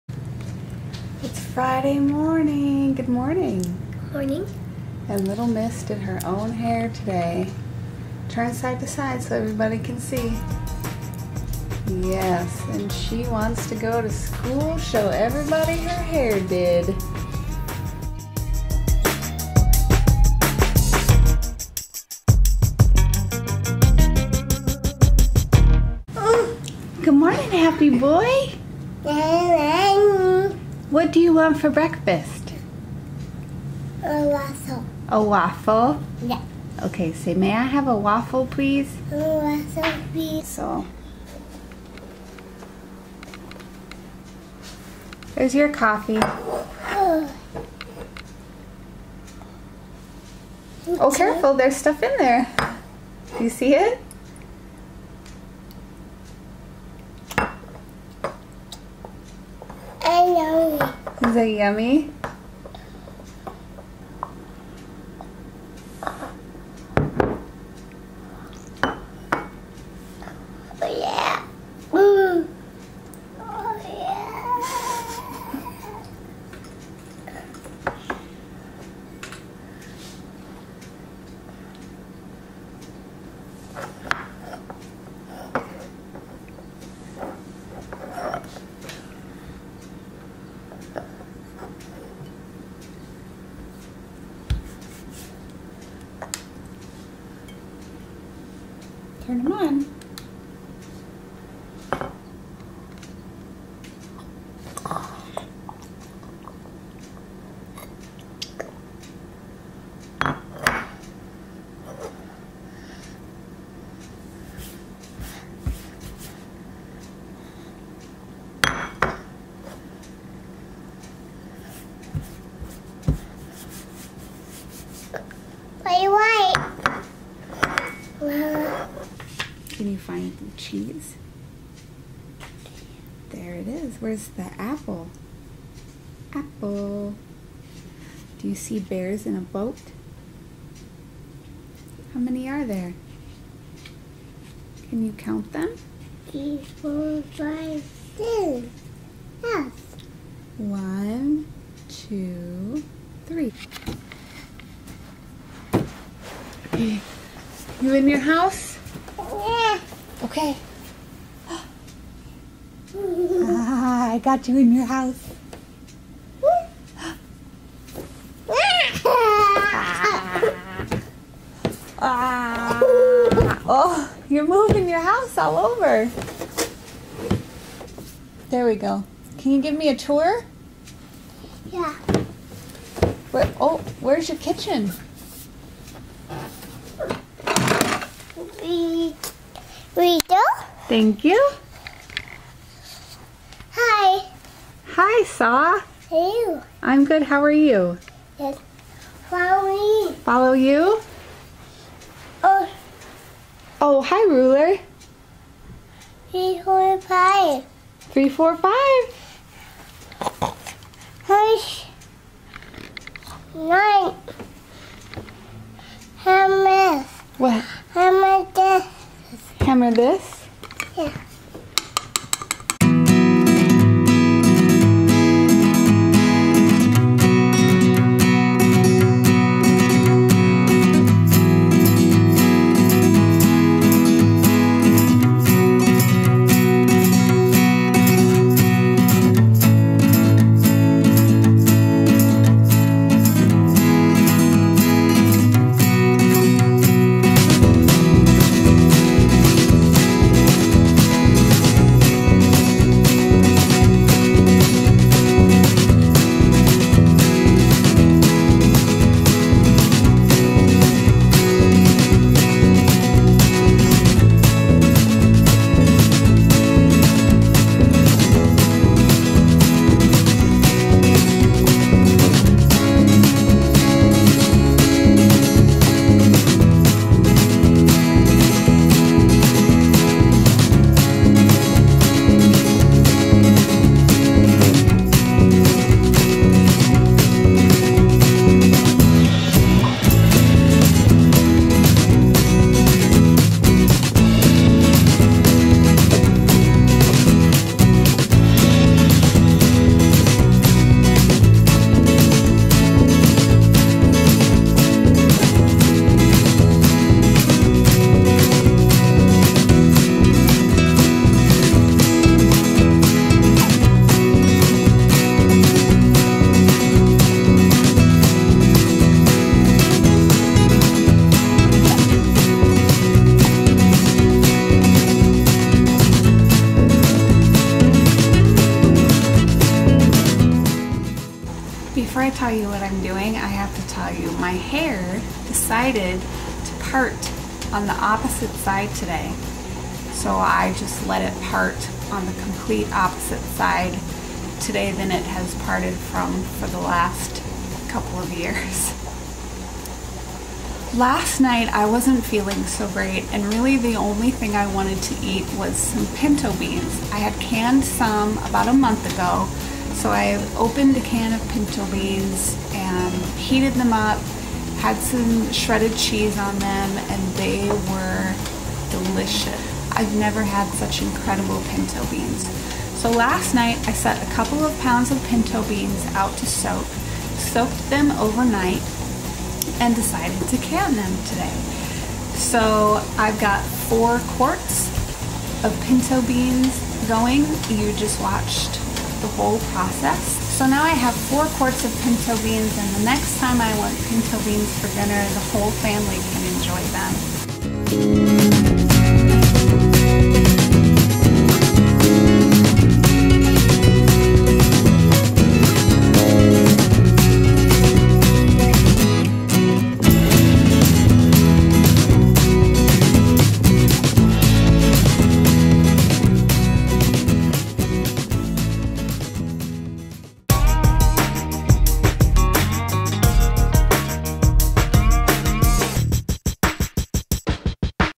Little Apartment On The Prairie: Friday Bean Day
Typical Friday morning for us, but Little Miss stepped up her hair game, my hair went a little rogue, and we got some more beans canned for future deliciousness!